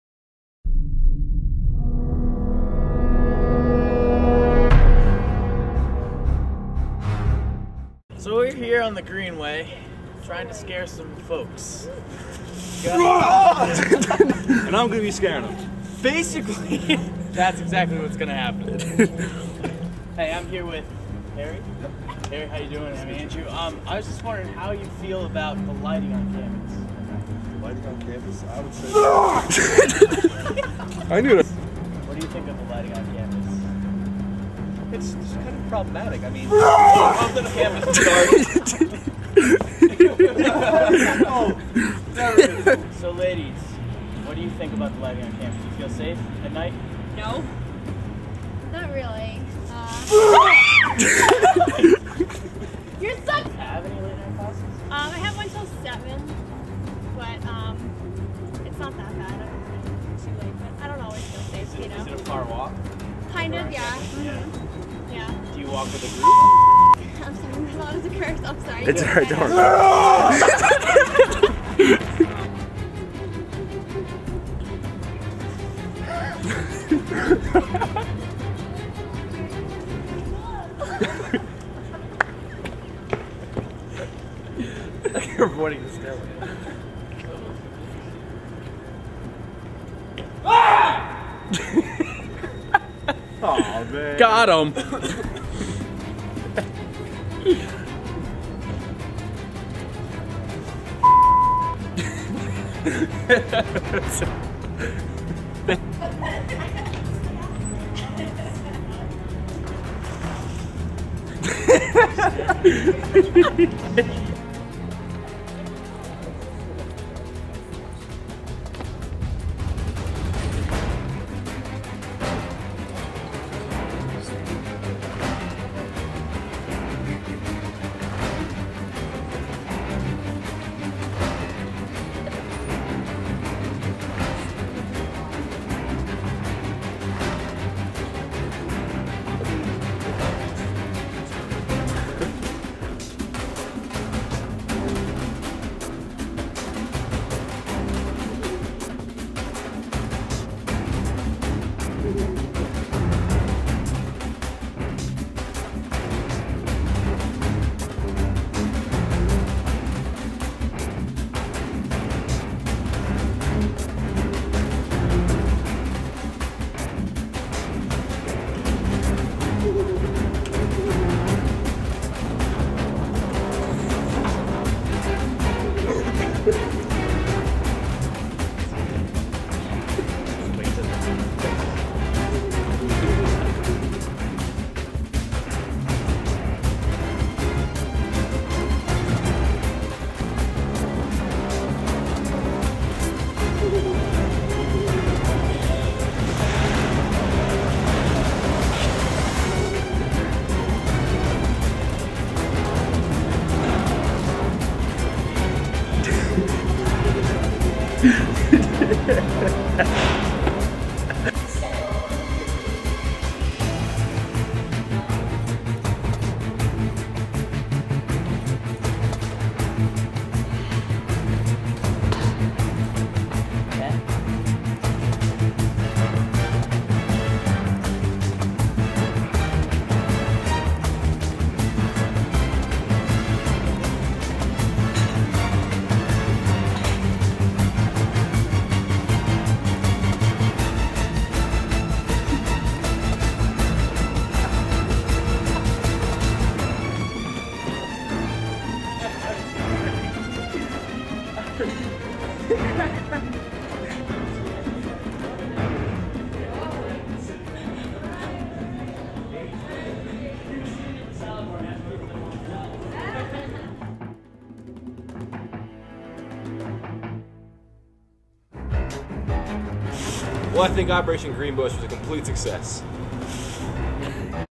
Wilkes University Bush Man
When the infamous Green Bush strikes Wilkes University, then Operation Green Bush is in full affect. Yeah, you know you're scared of a bush.